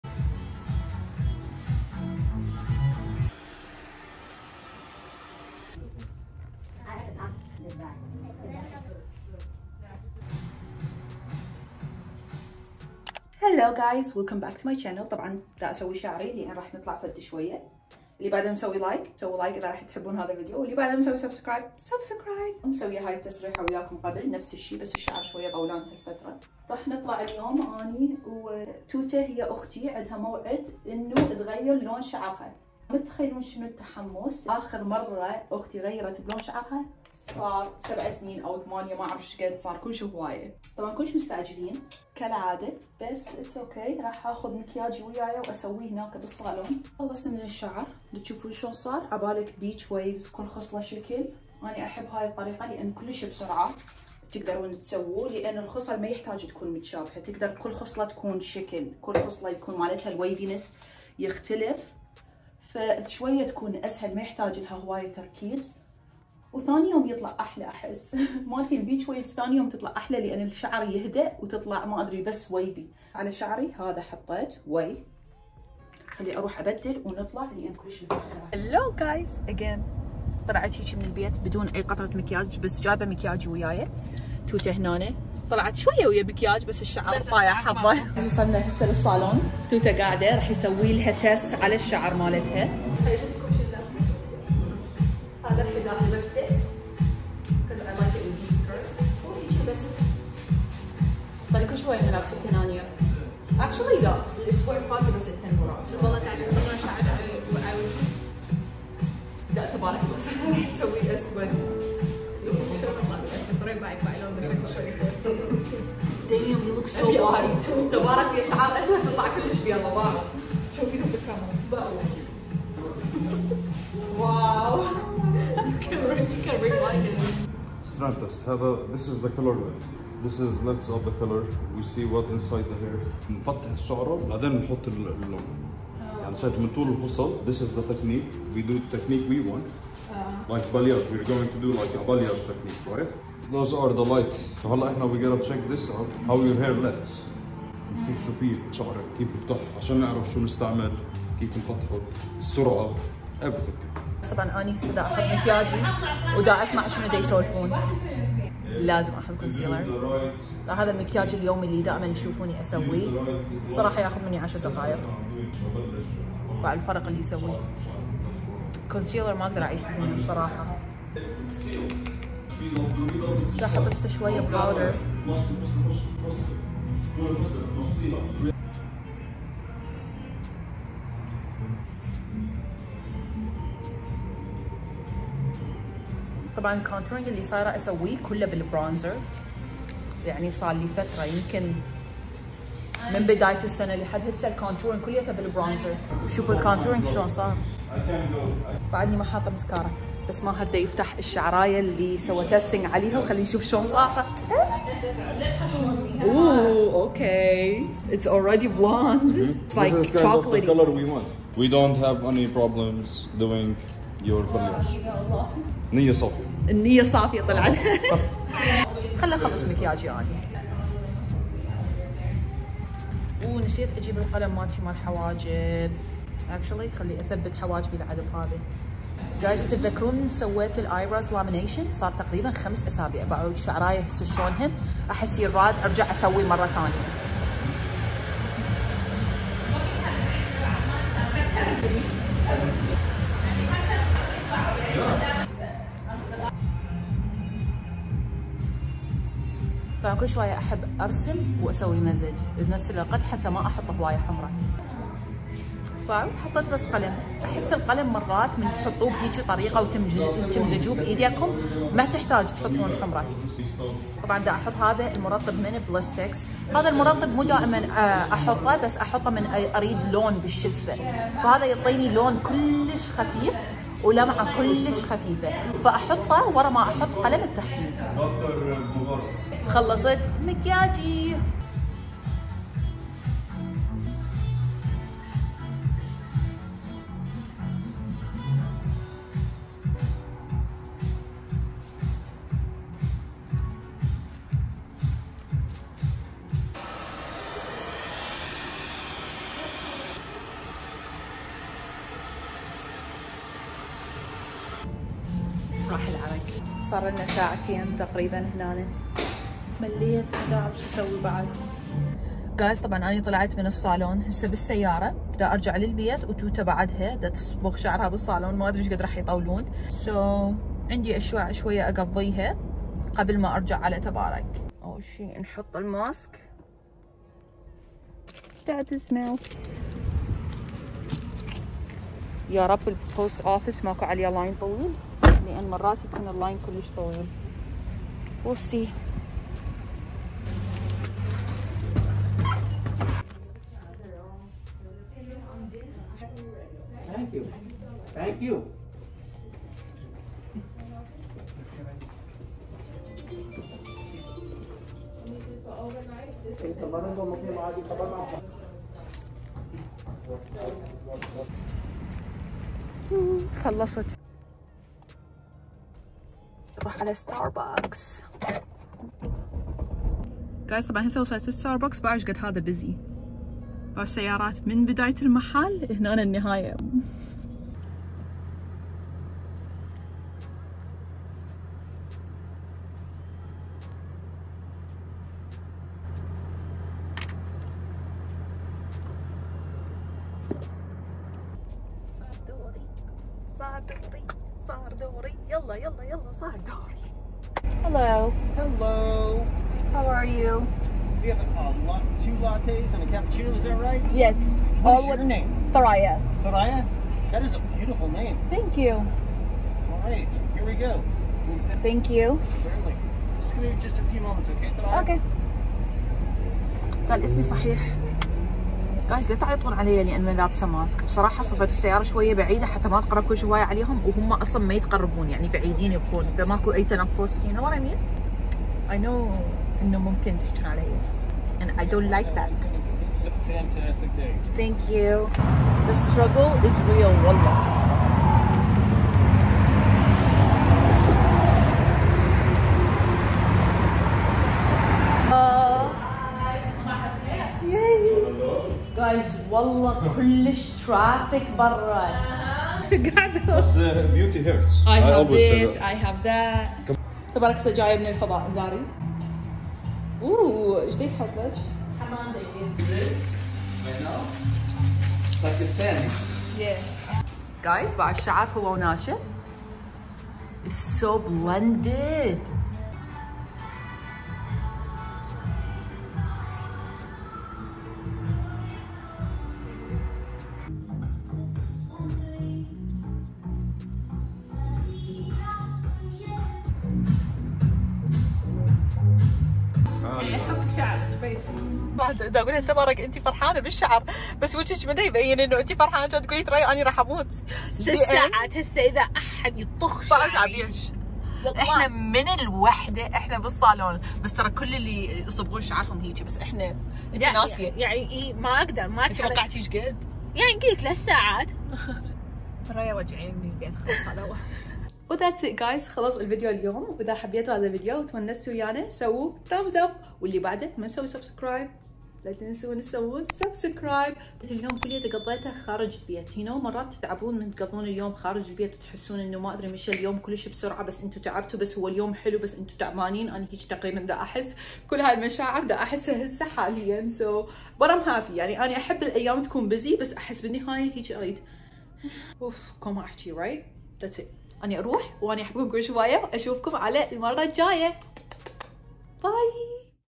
فلوك يومي| اختي صبغت شعرها لاول مرة بحياتها
هلووو كايز🥰🥰🥰
فديو جديييد اتمنى تتونسون ويانة
احبكم كولش هواية و اشوفكم عالمرة الجايه❤️

Amazon Store: